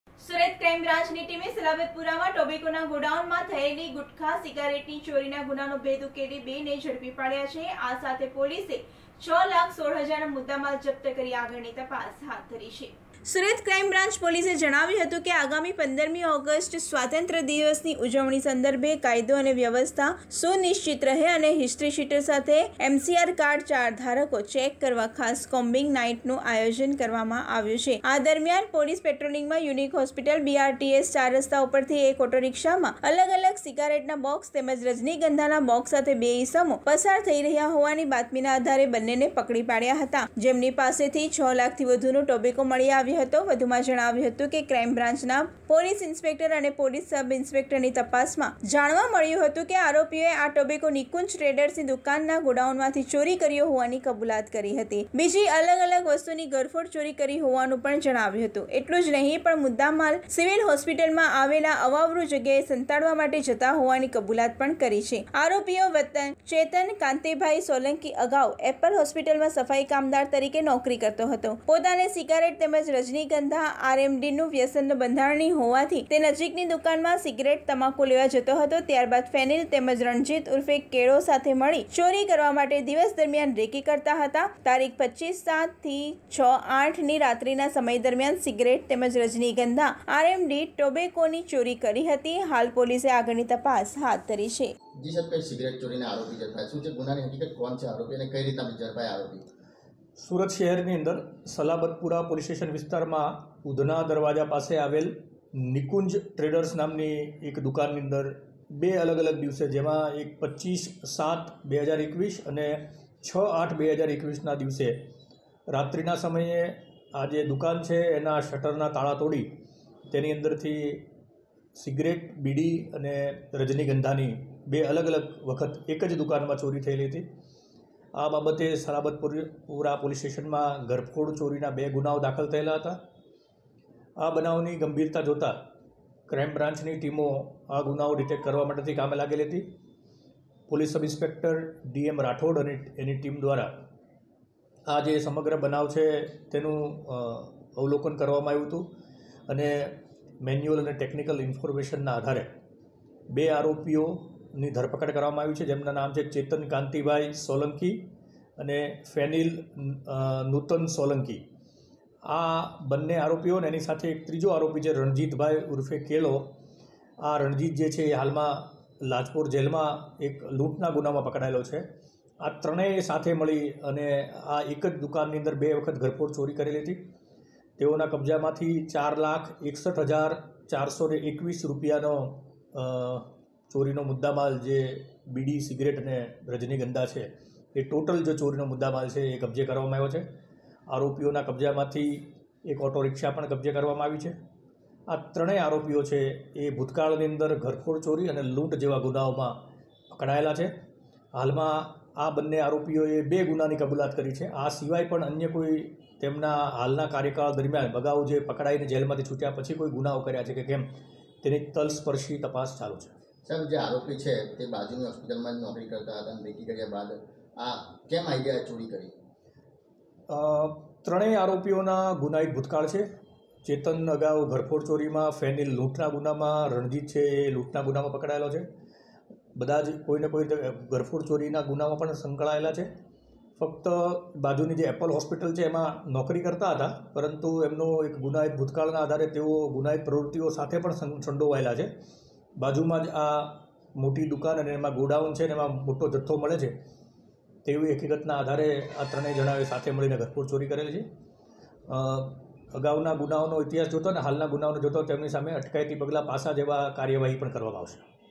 સુરતમાં સલાબતપુરામાં ટોબેકોના ગોડાઉનમાં થયેલી ચોરીમાં બે ઝડપાયા, 6.16 લાખનો |TV9INDIA GUJARATI
સુરત ક્રાઈમ બ્રાન્ચ પોલીસે જણાવ્યું હતું કે, આગામી 15 મી ઓગસ્ટ સ્વાતંત્ર્ય દિવસની ઉજવણી સંદર્ભે કાયદો અને વ્યવસ્થા સુનિશ્ચિત રહે અને હિસ્ટ્રી શીટર સાથે એમ.સી.આર કાર્ડ ધારકો ચેક કરવા ખાસ કોમ્બિંગ નાઈટનુ આયોજન કરવામાં આવ્યું છે. એ દરમિયાન પોલીસ પેટ્રોલિંગમાં યુનિક હોસ્પિટલ બી.આર.ટી.એસ ચાર રસ્તા ઉપરથી એક ઓટો રિક્ષા (GJ-05-BW-9812)માં અલગ-અલગ સિગારેટના બોક્ષ તેમજ રજનીગંધાના બોક્ષ સાથે બે ઈસમો પસાર થઈ રહ્યા હોવાની બાતમીના આધારે પોલીસે બન્નેને પકડી પાડ્યા હતા. જેમની પાસેથી પોલીસને 6 લાખથી વધુનો ટોબેકો મળી આવ્યો હતો.વધુમાં જણાવ્યું હતું કે, ક્રાઈમ બ્રાંચના પોલીસ ઇન્સ્પેક્ટર અને પોલીસ સબ ઈન્સ્પેકટની તપાસમાં જાણવા મળ્યું હતું કે આરોપીઓએ આ ટોબેકો નિકુંજ ટ્રેડસની દુકાનના ગોડાઉનમાંથી ચોરી કર્યો હોવાની કબૂલાત કરી હતી. બીજી અલગ-અલગ વસ્તુની ઘરફોડ ચોરી કરી હોવાનું પણ જણાવ્યું હતું. એટલું જ નહીં પણ મુદ્દામાલ સિવિલ હોસ્પિટલમાં આવેલા અવાવરૂ જગ્યાએ સંતાડવા માટે જતા હોવાની કબૂલાત પણ કરી છે. આરોપીઓ ચેતન કાંતીભાઇ સોલંકી અગાઉ એપ્પલ હોસ્પિટલમાં સફાઈ કામદાર તરીકે નોકરી કરતો હતો. પોતાને સીગારેટ તેમજ રજનીગંધા આર.એમ.ડીનું વ્યસનનો બંધારણી હોવાથી તે નજીકની દુકાનમાં સિગરેટ-તમાકુ લેવા જતો હતો. ત્યારબાદ ફેનીલ તેમજ રણજીત ઉર્ફે કેળો સાથે મળી ચોરી કરવા માટે દિવસ દરમિયાન રેકી કરતા હતા. તા-25/7/2021 તેમજ 6/8/2021ની રાત્રીના સમય દરમિયાન સીગારેટ તેમજ રજનીગંધા, આર.એમ.ડી ટોબેકોની ચોરી કરી હતી. હાલ પોલીસે આગળ ની તપાસ હાથ ધરી છે.